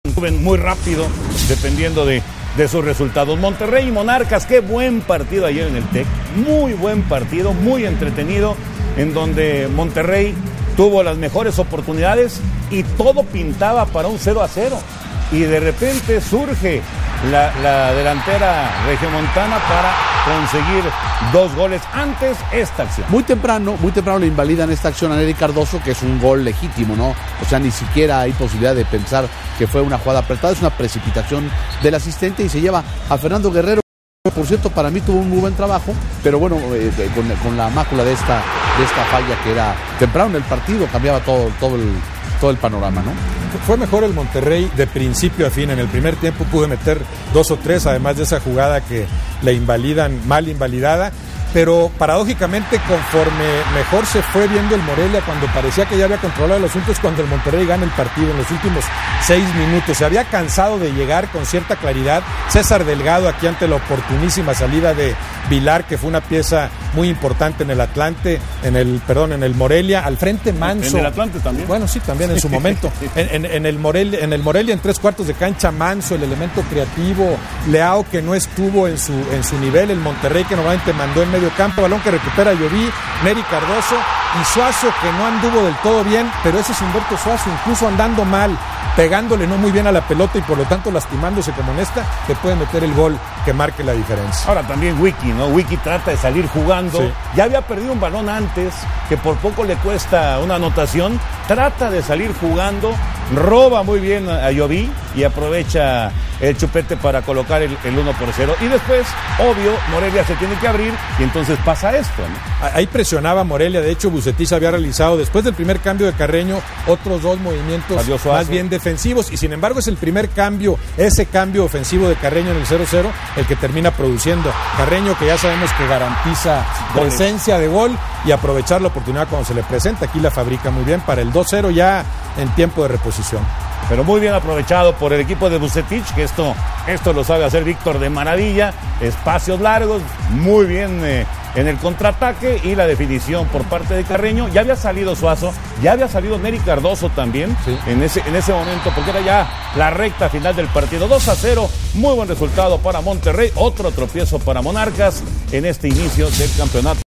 Monterrey - Morelia (Jornada 4 Apertura 2011)
Sábado 06 de agosto de 2011 a las 21:00 hrs.Estadio Tecnológico
Goles:1 - 0 Humberto Suazo al minuto 85' Humberto Suazo hizo el que mueve los cartones en el Tecnológico. Se mete al área entre defensores purépechas y con punterazo manda la pelota al fondo de las redes.
2 - 0 Abraham Darío Carreño al minuto 91'